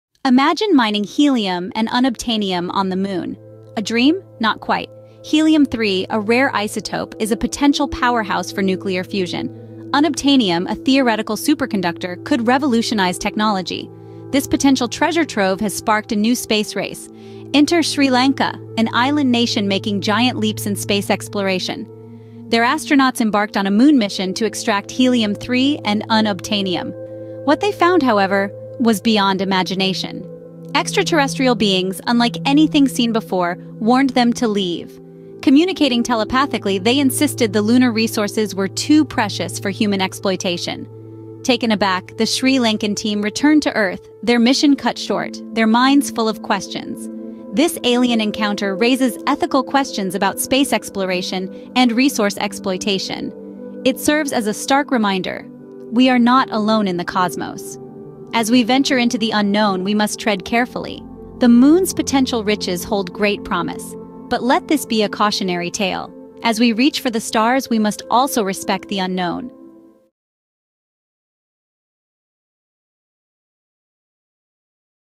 Moon Mining - helium & Unobtanium සඳ මත හීලියම් කැනීමේදි පිටසක්වල ජීවීන්ගෙන් අනතුරු ඇඟවීමක්.
Moon Mining_ A Cautionary Tale .mp4